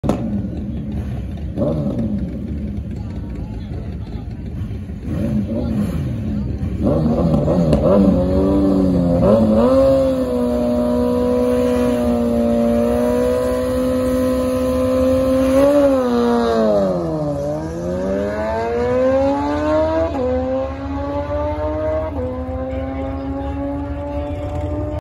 Kawasaki Z900RS at the drag strip
Running the 900RS at Pacific Raceways.
11.50 ET at 114mph